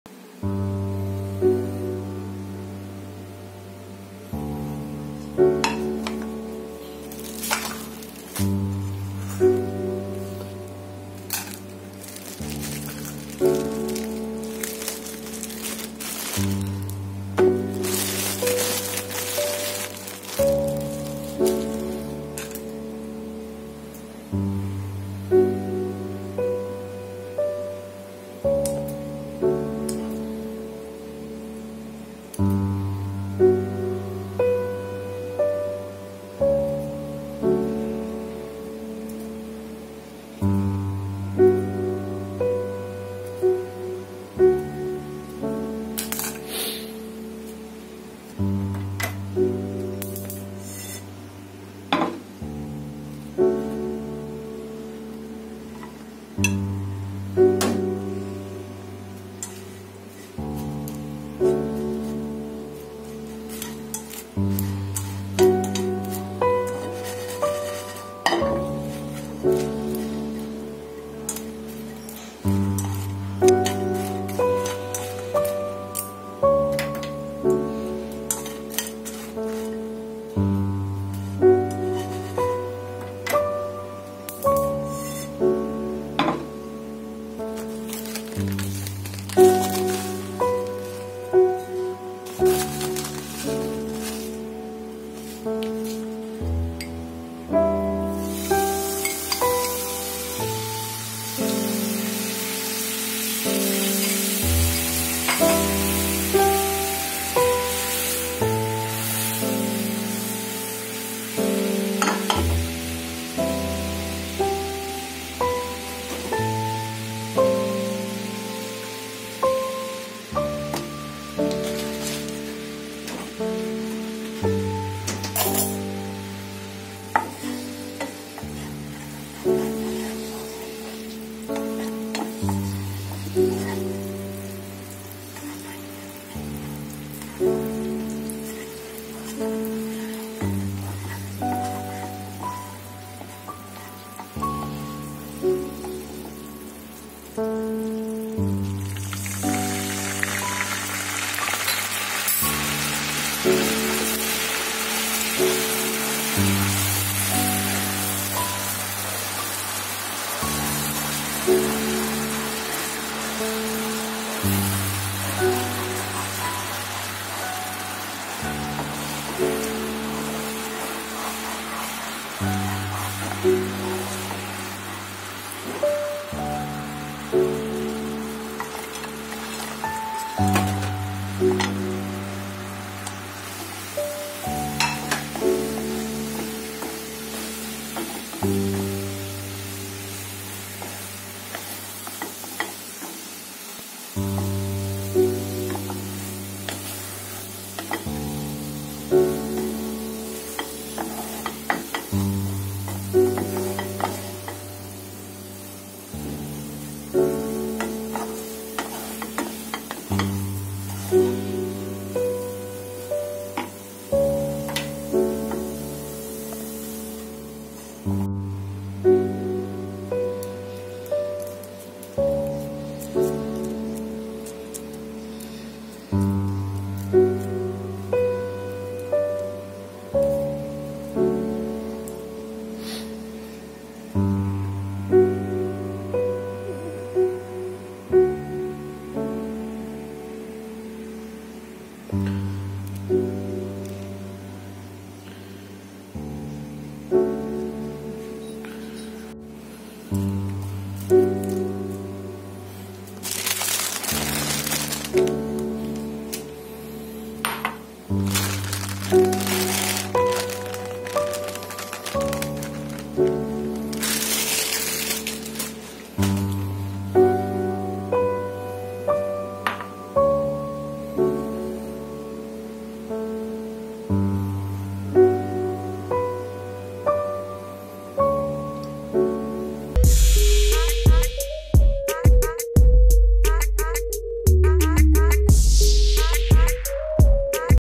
CARAMEL BREAD POPCORN | PERSIS KAYAK DI XXI
Ini adalah video proses pembuatan caramel bread pop corn, rasanya ini miriiiiip banget sama caramel pop corn yang ada di XXI lho! buat kalian yang kangen sama snack XXI bisa banget nih buat bkin caramel bread pop corn ini dirumah sendiri yaa! silahkan dicoba ya guys, dijamin nagih bangeeet. see you, enjoy~